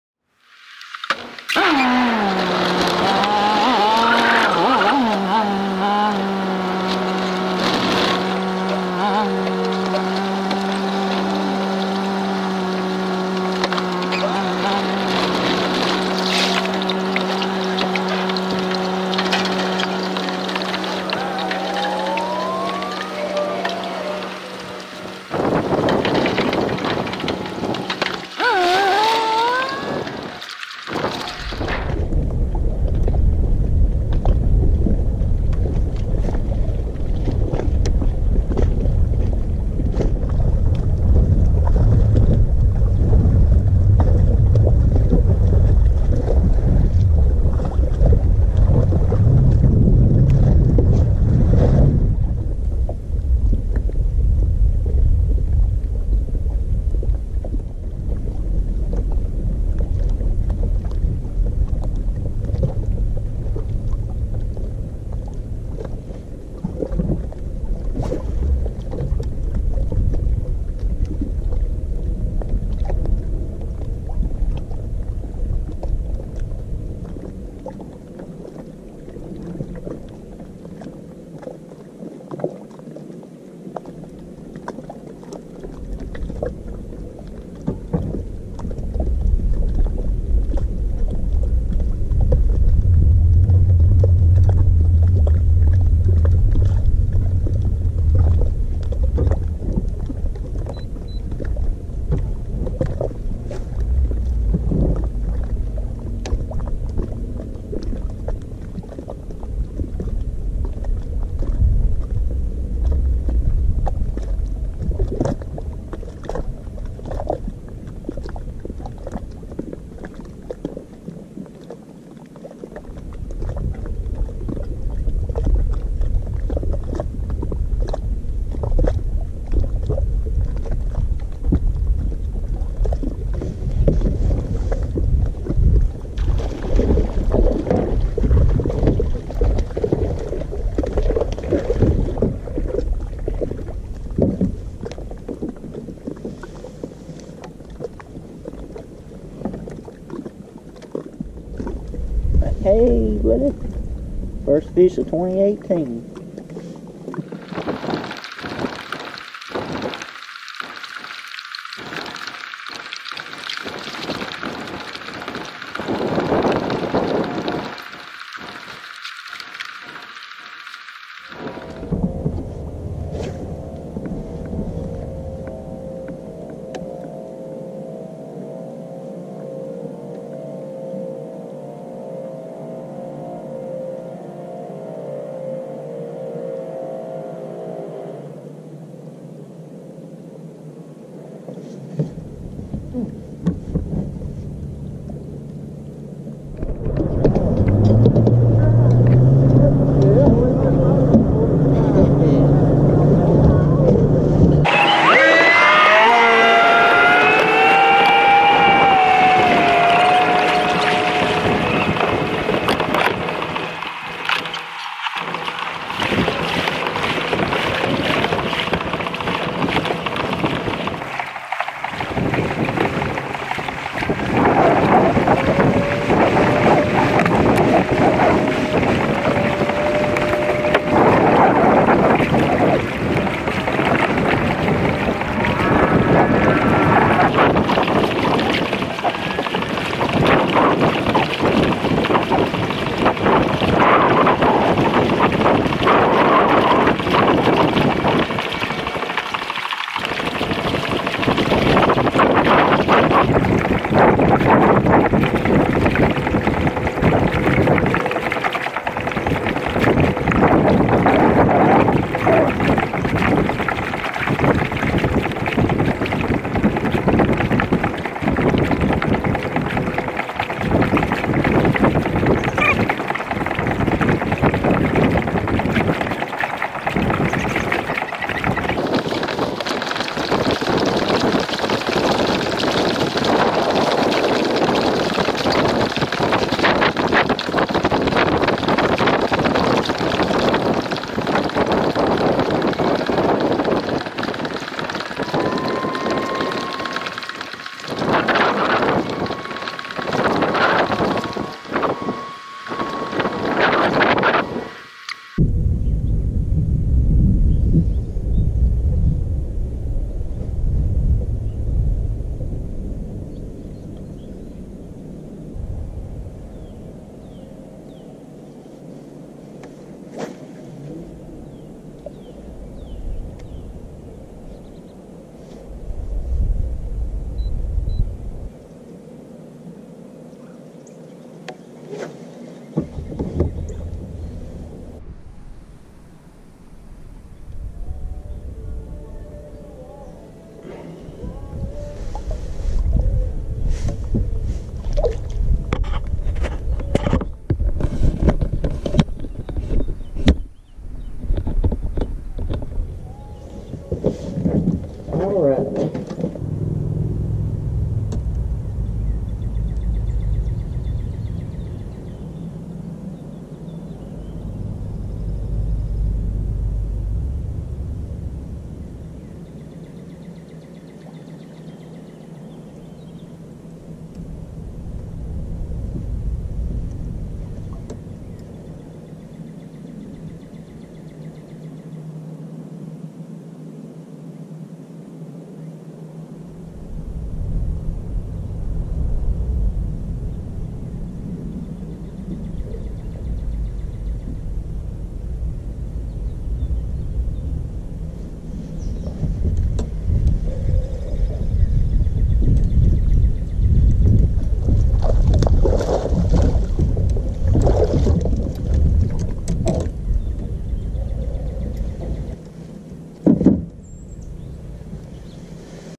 New kayak, Prefishing, and First Tournament
testing new kayak, first fish of 2k18, and tournament on lake Keowee. I couldn't go back in and edit this video so this is all I could publish. still trying to learn how to use iMovie.